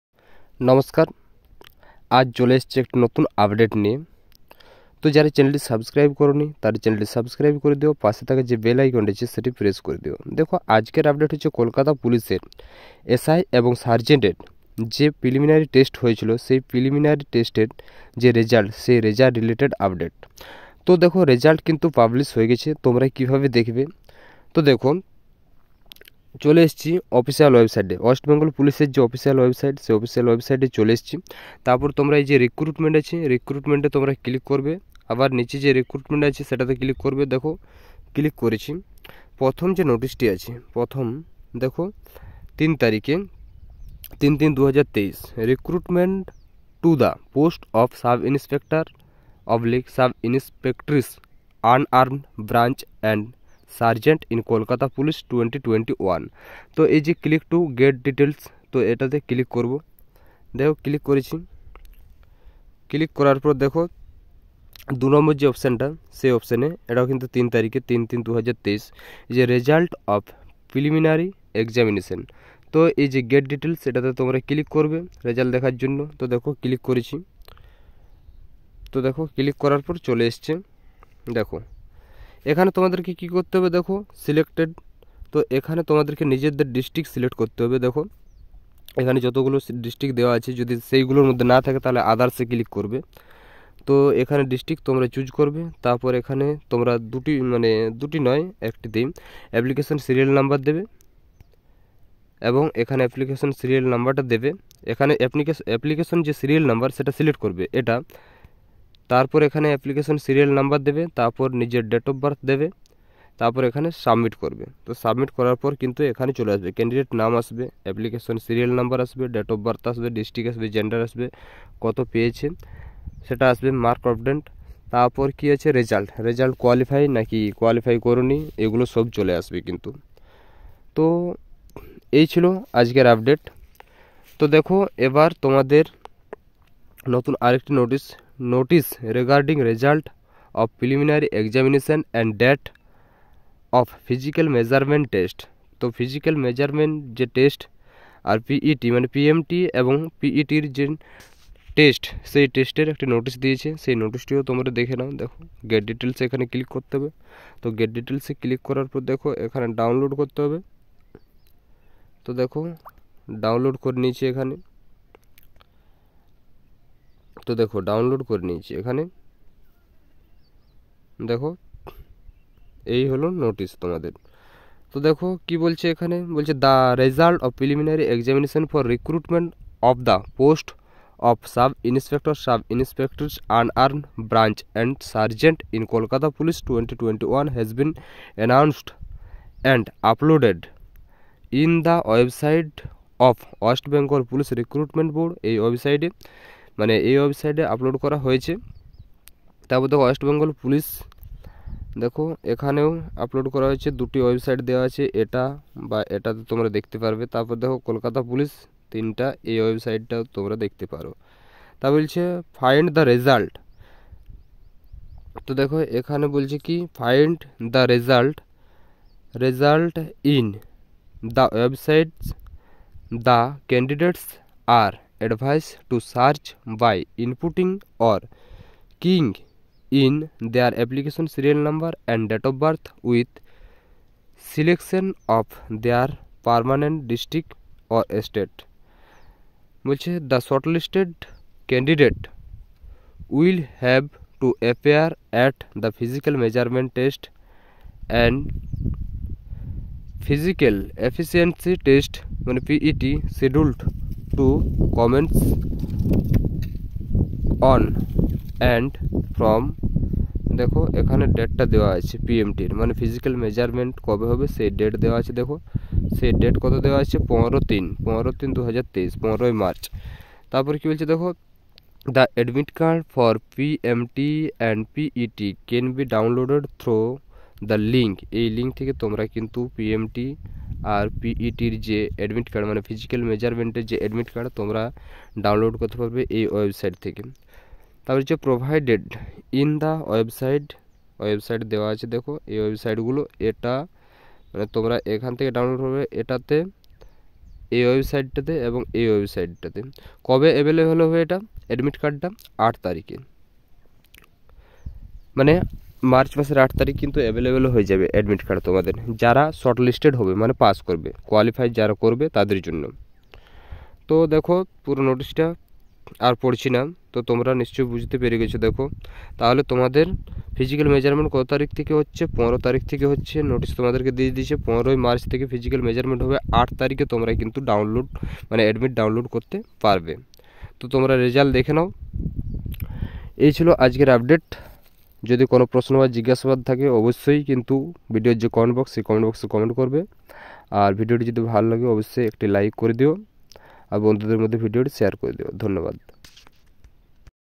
Preliminary result Published !! PMT/PET Schedule !! SI and Sargent in Kolkata Police-2021!!
RECRUITMENT TO THE POSTS OF SUB
-INSPECTOR/SUB-INSPECTRESS
(UN-ARMED BRANCH) AND SERGEANT IN KOLKATA POLICE
 - 2021

The result of Preliminary Examination for recruitment to the post of SubInspector/Sub-Inspectress (Un-Armed Branch) and Sergeant in Kolkata Police -
2021 has been announced and uploaded in the websites of West Bengal Police
Recruitment Board
find the result in the websites the candidates are advised to search by inputting
or keying-in their Application Sl. No. and Date of Birth with selection of their
permanent District/State. The shortlisted candidates will have to appear at the
Physical Measurement Test (PMT) and Physical Efficiency Test (PET) scheduled
to commence on and from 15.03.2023. The Admit Cards for PMT & PET can be
downloaded through the link (wbprb.applythrunet.co.in) provided in the
websites of West Bengal Police Recruitment Board and
West Bengal Police from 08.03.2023.
The candidates shall have to produce the printout of their admit card to get
admittance into the PMT & PET arena. The candidates applied online will not get
any paper admit card which shall be issued only in respect of offline candidates
(by post only). Candidates are advised to follow the instructions on their admit card and visit the above-mentioned websites for further update.